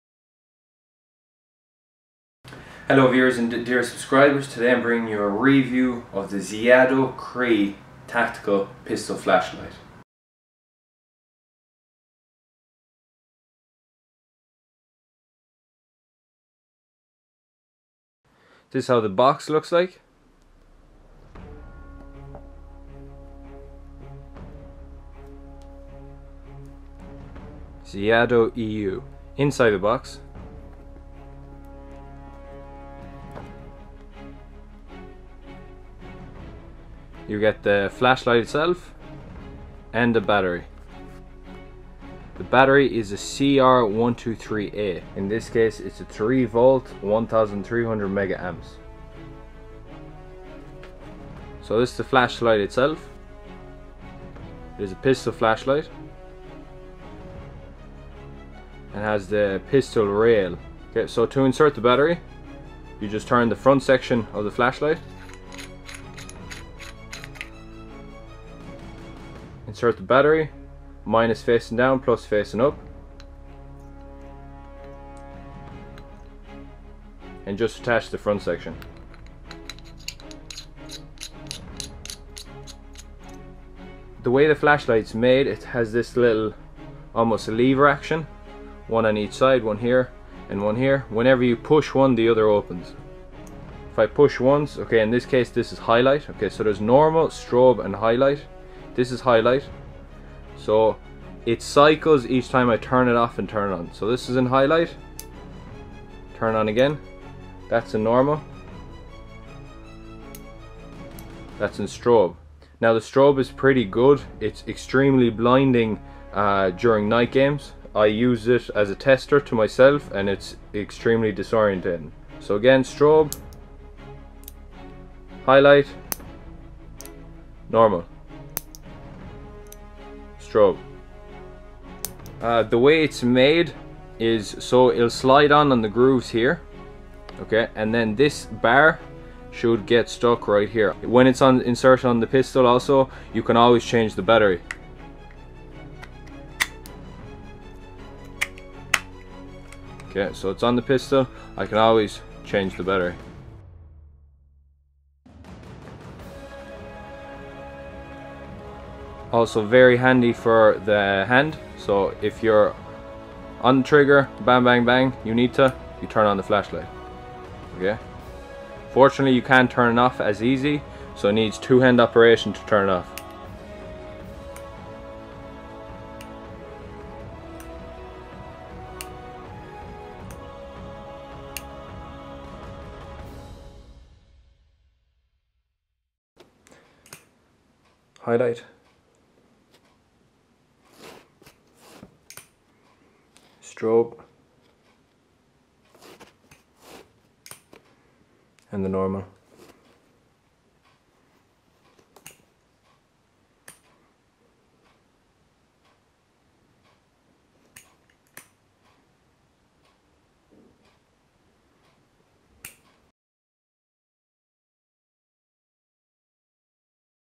Zeadio CREE LED Pistol Flashlight Review
Features:
Modes: Normal, Strobe, Highlight
Mount: Picatinny/Weaver Adapter, Light Rail Mount
Battery: 1x CR123A
Length: 76mm, Height: 40mm, Width: 26mm
Weight: 82g
Material: Aluminium